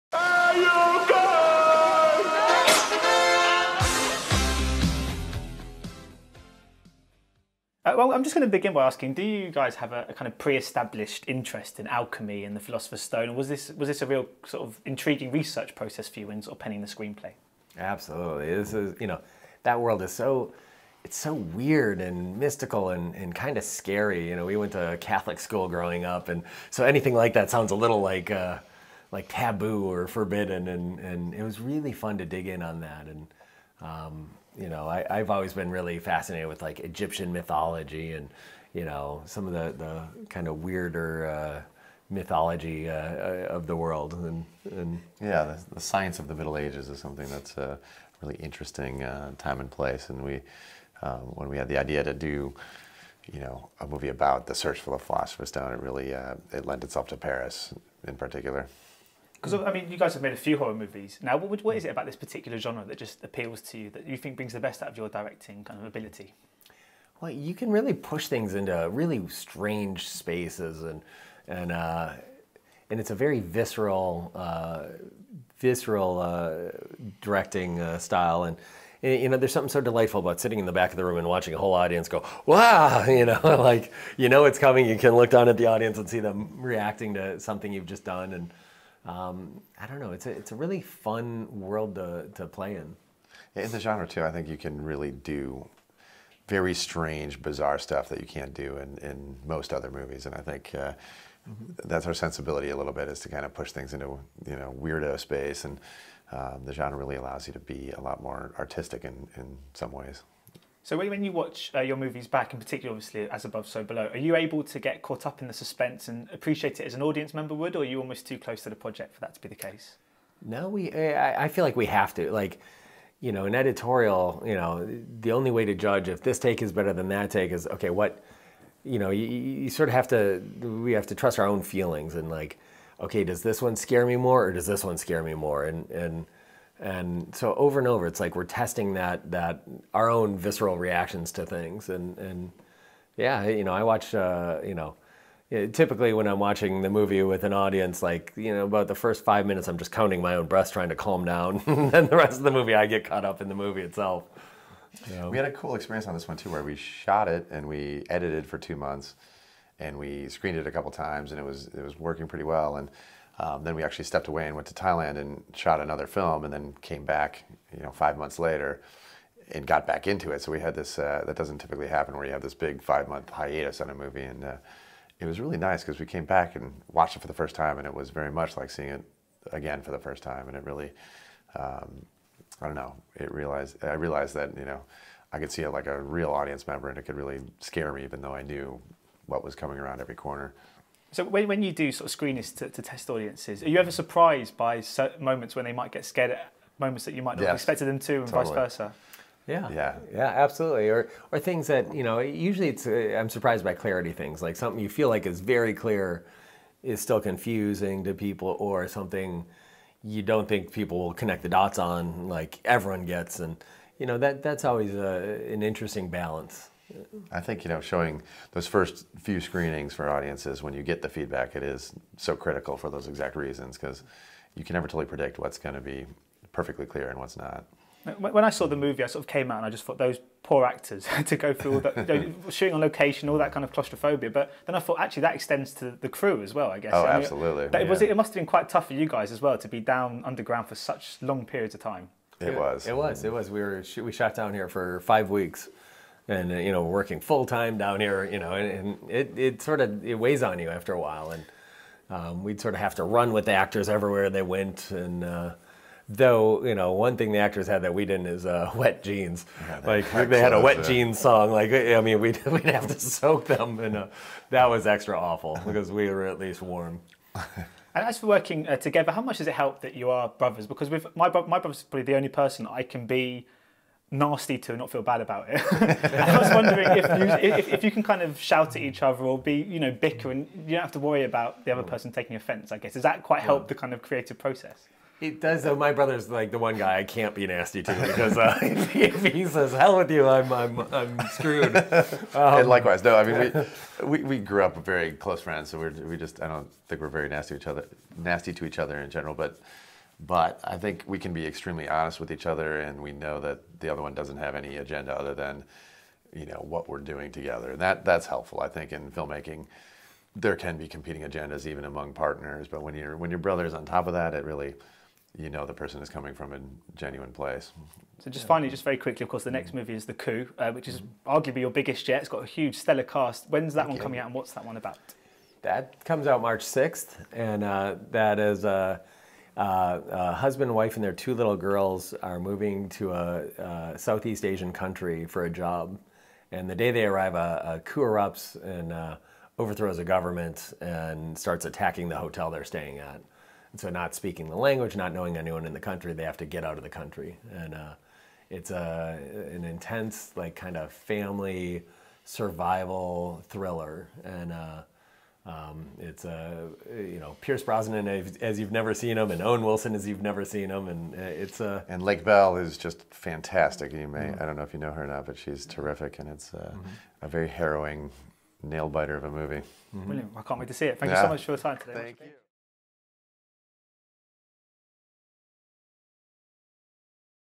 John Erick Dowdle and Drew Dowdle Interview - As Above, So Below
Stefan Pape interviews Director and Writers John Erick Dowdle and Drew Dowdle for their movie So Above, So Below.